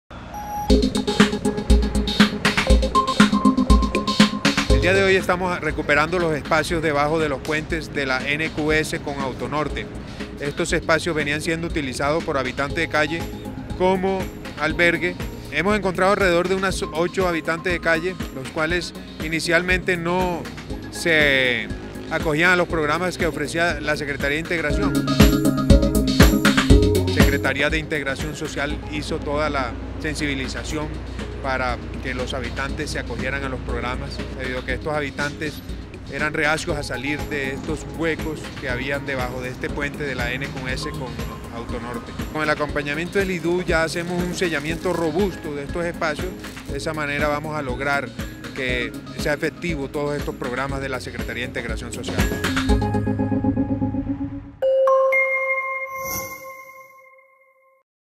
Distrito recupera el puente vehicular de la Calle 92 con Avenida NQS
La Alcaldía Local de Chapinero recuperó este martes el puente vehicular de la calle 92 con Avenida NQS – Autopista Norte, que era ocupado ilegalmente. El Distrito espera que las personas que habitaban al interior de la estructura y a sus costados acepten las alternativas de atención integral dispuestas para ellos.